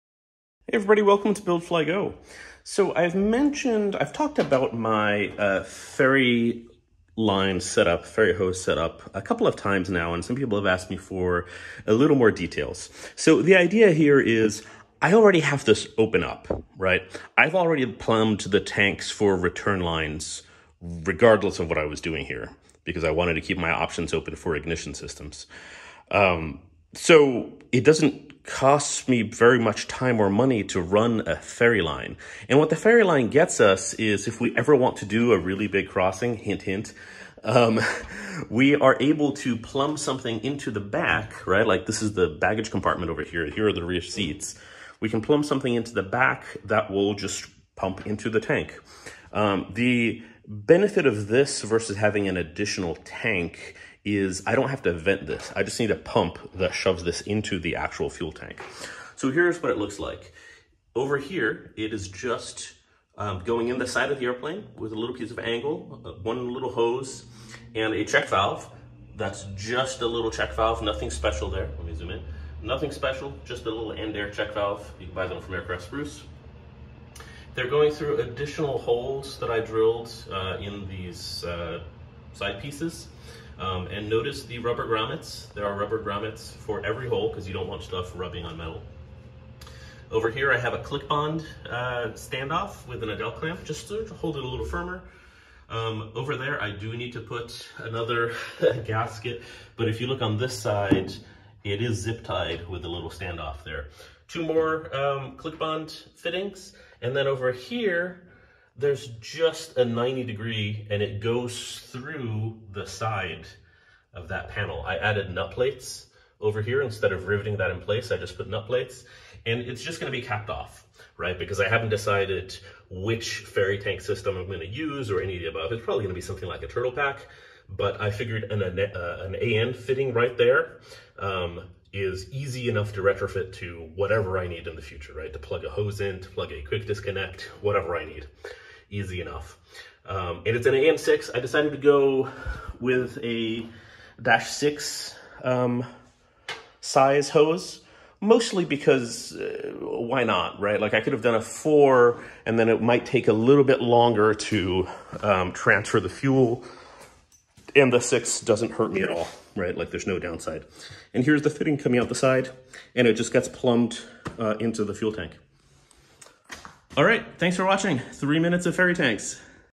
RV-10 Mod: Ferry Tank Plumbing Overview
Viewer request video: Quick Overview of our ferry tank hose setup